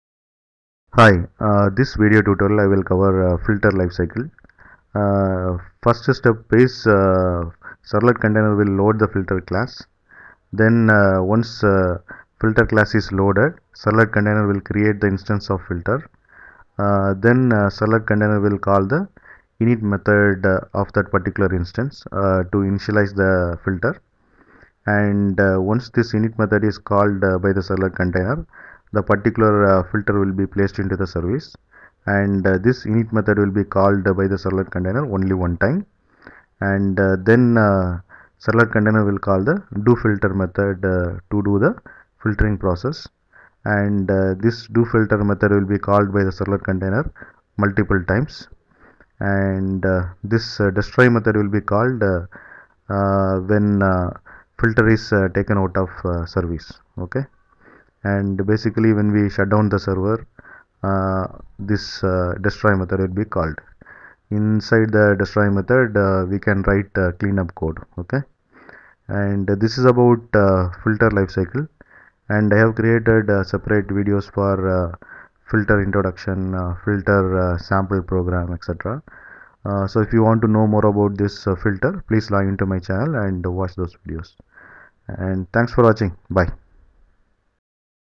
What is Filter Lifecycle? | Servlets Filter Lifecycle: What You Need to Know
In this tutorial, we'll delve into the concept of the filter lifecycle in servlets, exploring its significance and the sequence of events that occur during the lifecycle of a servlet filter. Understanding the filter lifecycle is essential for effectively implementing and managing servlet filters in your web applications.

Here's what you'll learn in this video:

1. **Introduction to Filter Lifecycle**: We'll start by providing a comprehensive overview of the filter lifecycle in servlets, explaining its role in the request processing pipeline. You'll understand how servlet filters are instantiated, initialized, and destroyed, and the stages they go through during their lifecycle.

2. **Key Phases of Filter Lifecycle**: We'll explore the key phases of the filter lifecycle, including initialization, request processing, and destruction. You'll learn about the methods defined in the javax.servlet.Filter interface and how they are invoked by the servlet container at each stage of the lifecycle.

3. **Order of Execution**: We'll discuss the order in which filters are invoked in the filter chain and how filter mappings determine the sequence of execution. You'll understand how multiple filters can be chained together to process requests and responses in a specific order.

4. **Lifecycle Events**: We'll examine the lifecycle events that trigger the execution of filter methods, such as servlet container initialization, request processing, and servlet container shutdown. You'll learn how to leverage these events to perform initialization tasks, request preprocessing, and resource cleanup.

5. **Practical Examples**: Using code snippets and examples, we'll demonstrate the filter lifecycle in action, illustrating the sequence of method invocations and their impact on request processing. You'll gain hands-on experience with implementing and managing servlet filters in real-world scenarios.

6. **Best Practices and Tips**: Throughout the tutorial, we'll share best practices and tips for effectively managing the filter lifecycle in your servlet-based projects. From ensuring proper resource cleanup to handling exceptions and errors gracefully, these insights will help you build robust and maintainable web applications.

By the end of this tutorial, you'll have a thorough understanding of the filter lifecycle in servlets and how to leverage it to enhance the functionality and performance of your web applications. Whether you're a beginner or an experienced developer, mastering the filter lifecycle is essential for building scalable and resilient servlet-based solutions.

So, join us as we explore the intricacies of the filter lifecycle in servlets!

What is Filter Lifecycle? | Servlets